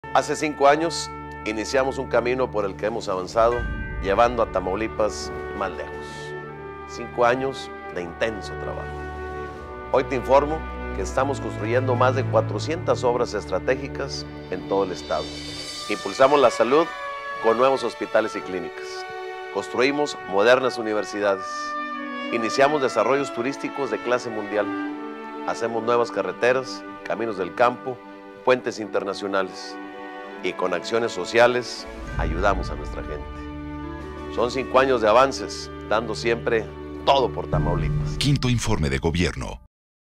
15-09-10 5to Informe de Gobierno Eugenio Hernández Flores
hace 5 años iniciamos un camino por el que hemos avanzado llevando a Tamaulipas mas lejos, 5 añso de intenso trabajo. Hoy te informo que estamos construyendo mas de 400 obras estratégicas en todo el estado.
impulsamos la salud con nuevos hospitales y clínicas, construimos modernas universidades, iniciamos desarrollos turísticos de clase mundial, hacemos nuevas carreteras, caminos del campo, puentes internacionales y con acciones sociales ayudamos a nuestra gente.

Son 5 años de avance dando todo por Tamaulipas.